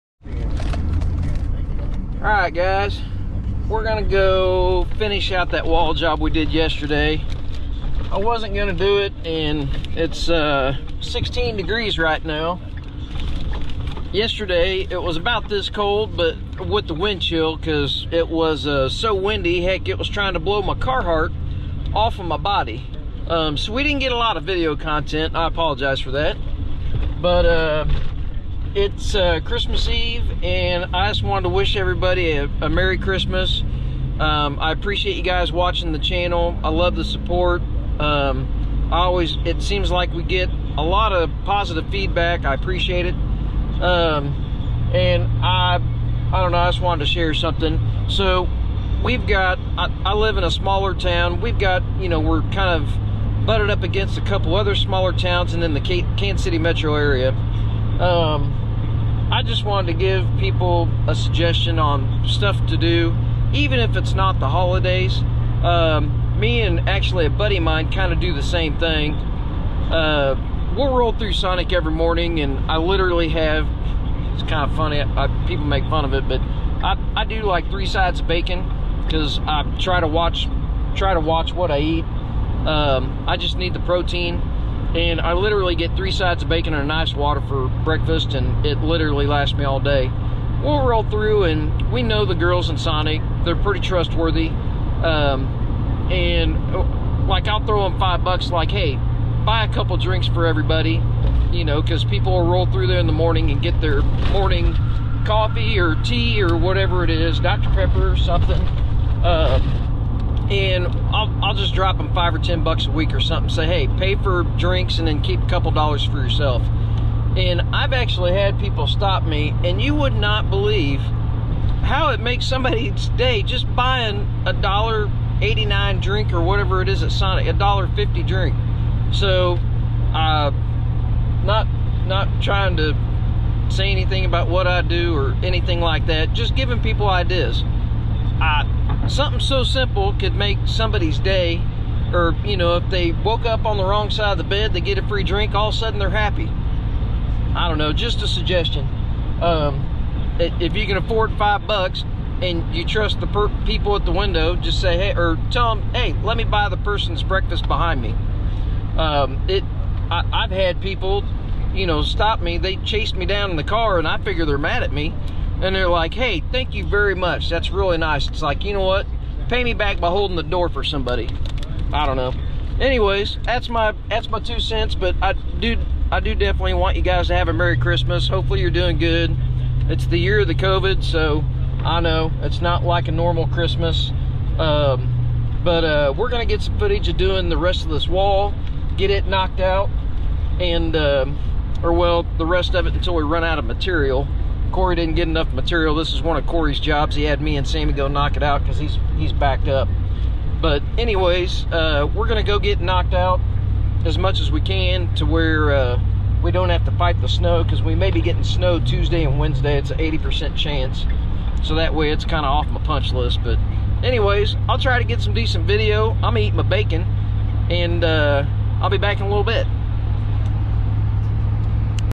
Merry Christmas from the Skid-Co channel. Try to make someones day.
It easy to change someones day. hold the door for them. Buy them a soda in the morning. If your in the drive thru somewhere, buy someones meal. say hello to them.

Merry Christmas everyone. I hope you are good and ready for the new year.